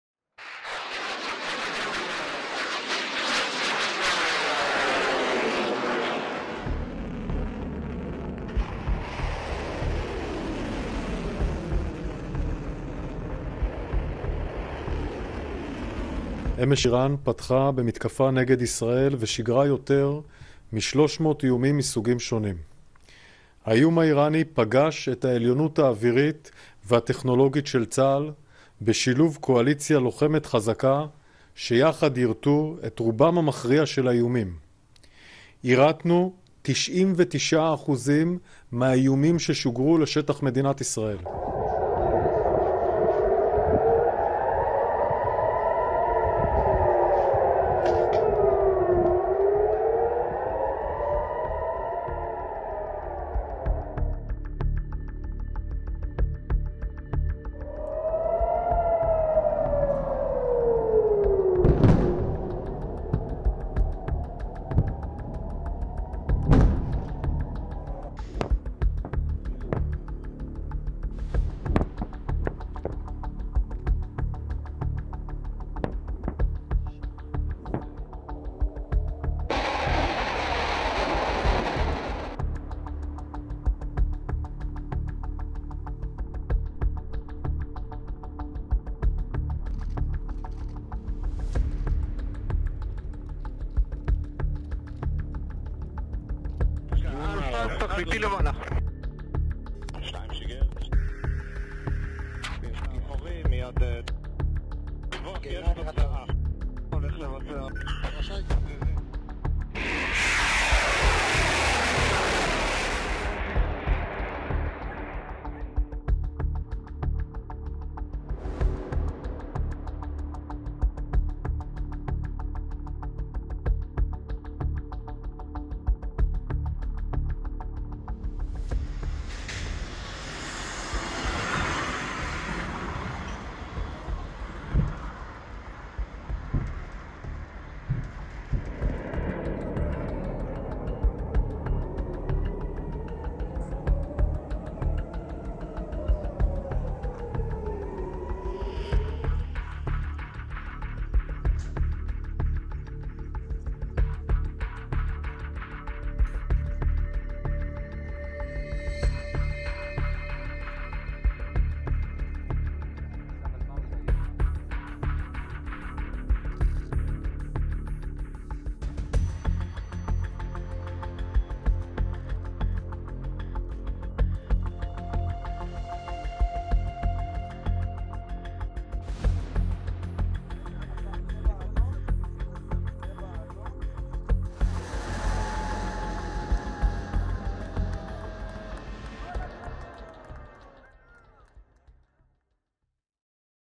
Explainer: The Iron Dome, Israel's key anti-missile shield • FRANCE 24 English
Israel's air defences were in operation on Saturday night, after Iran launched more than 300 drones and missiles in an unprecedented direct attack. That starts with the Iron Dome, which has intercepted thousands of rockets since it first went into operation in 2011, provides Israel with crucial cover during times of conflict. It has been heavily relied upon to protect military and civilian sites from frequent barrages of rockets fired from Gaza and Lebanon in the ongoing war between Israel and Hamas.